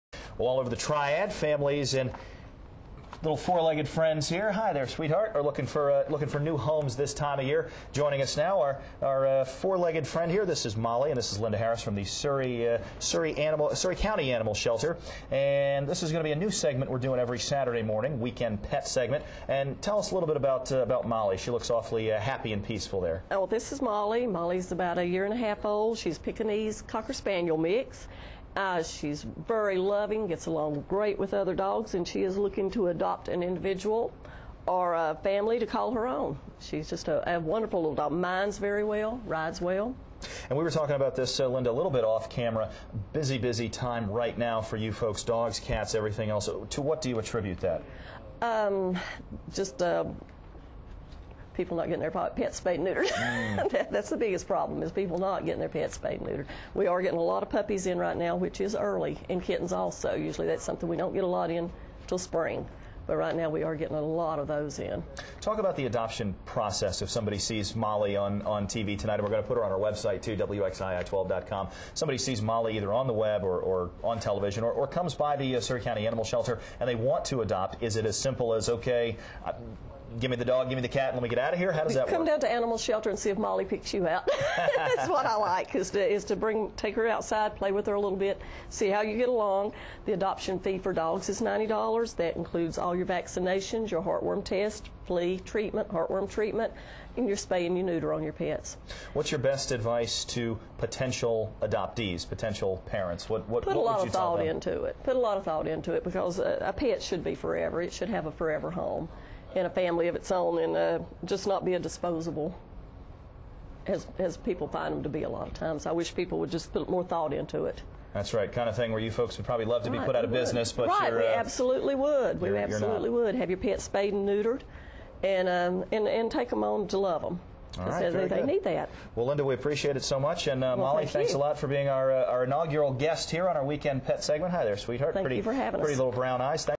Pet Of The Week - Molly
Meet Molly, WXII 12's Pet of the Weekend.  Linda Harris from the Surry County Animal Shelter has more.